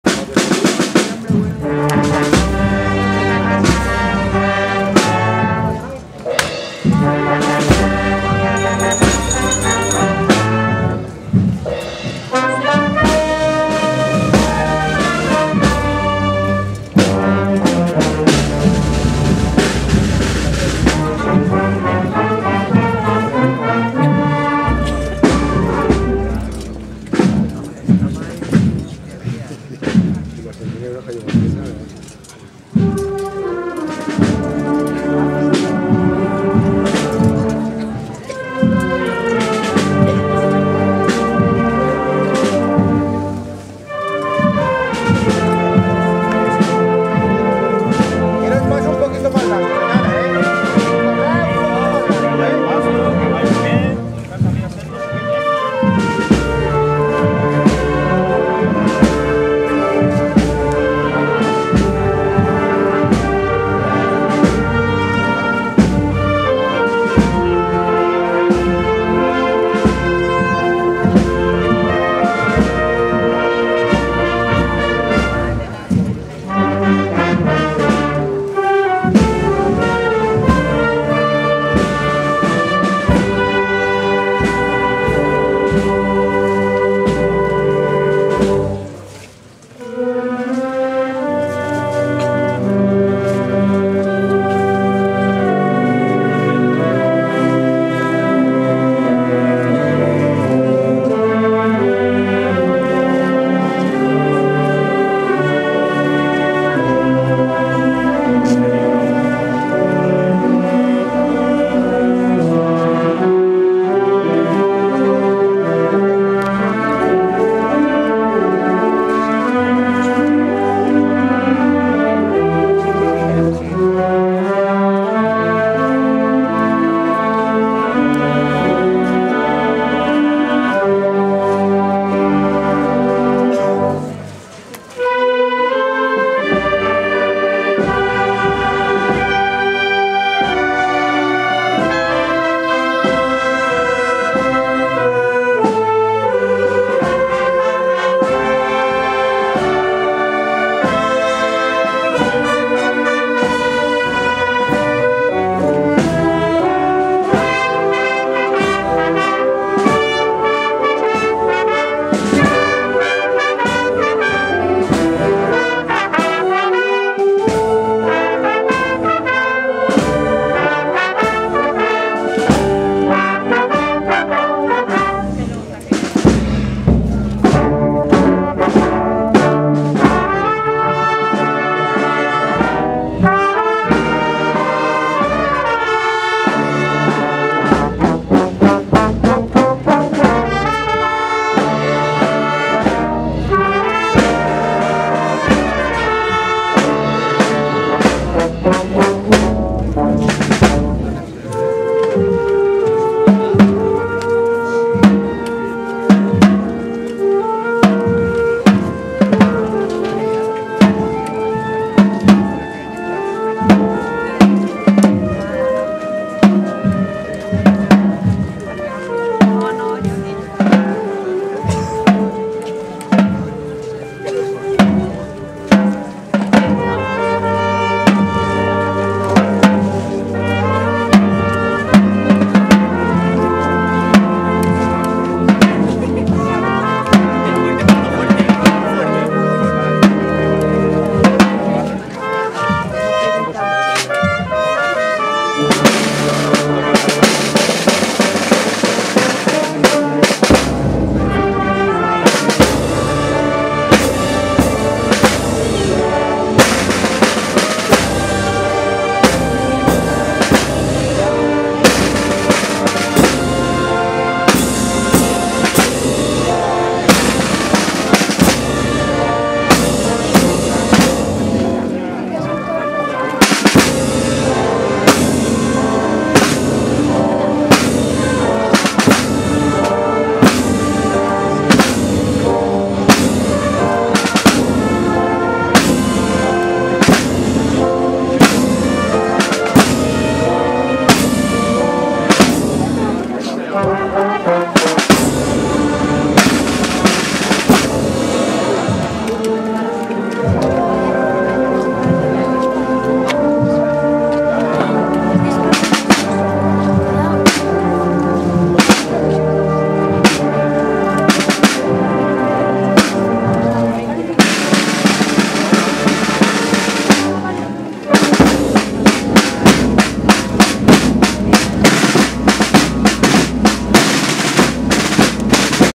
Mayor Dolor - Virgen del Calvario - Doña Mencía 2019
Cofradía Obrera del Santísimo Cristo del Calvario, María Santísima del Mayor Dolor, San Juan Evangelista y Santa María Magdalena.
Banda Municipal de Música "Jose María Gómez Caballero"
Viernes Santo del año 2019, Doña Mencía (Córdoba)